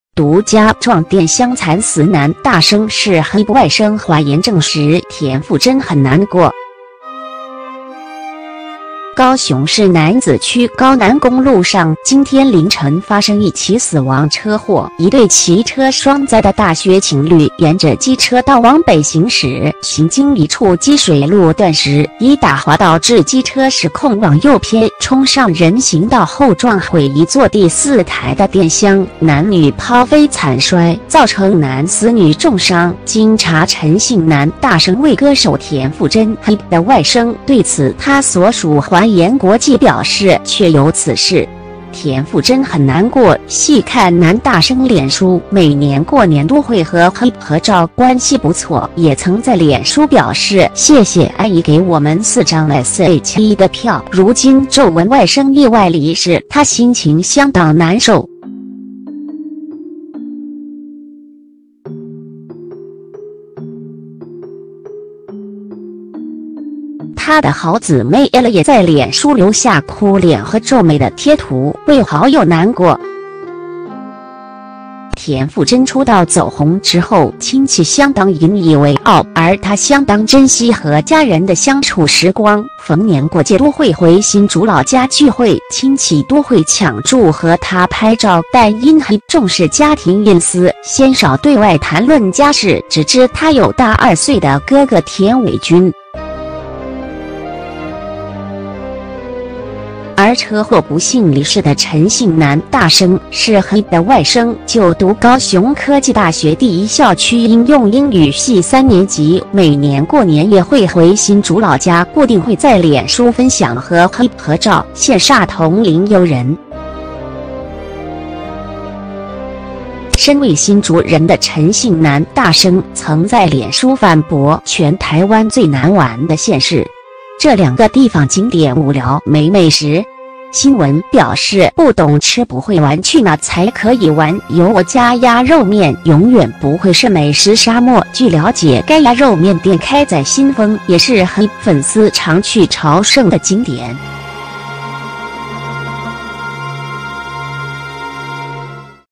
【獨家】撞電箱慘死男大生是Hebe外甥 華研證實「田馥甄很難過」
高雄市楠梓區高楠公路上，今天凌晨發生一起死亡車禍，一對騎車雙載的大學情侶，沿著機車道往北行駛，行經一處積水路段時，疑打滑導致機車失控往右偏，衝上人行道後撞毀一座第四台的電箱，男女拋飛慘摔，造成男死女重傷，經查陳姓男大生為歌手田馥甄（Hebe)的外甥，對此她所屬華研國際表示「確有此事，田馥甄很難過」，細看男大生臉書，每年過年都會和Hebe合照，關係不錯，也曾在臉書表示「謝謝阿姨給我們4張S.H.E的票」，如今驟聞外甥意外離世，她心情相當難受。她的好姊妹Ella也在臉書留下哭臉和皺眉的貼圖，為好友難過。
田馥甄出道走紅之後，親戚相當引以為傲，而她相當珍惜和家人的相處時光，逢年過節都會回新竹老家聚會，親戚都會搶著和她拍照，但因Hebe重視家庭隱私，鮮少對外談論家事，只知她有大2歲的哥哥田緯均。而車禍不幸離世的陳姓男大生，是Hebe的外甥，就讀高雄科技大學第一校區應用英語系3年級，每年過年也會回新竹老家，固定會在臉書分享和Hebe合照，羨煞同齡友人。
身為新竹人的陳姓男大生，曾在臉書反駁「全台灣最難玩的縣市！這兩個地方景點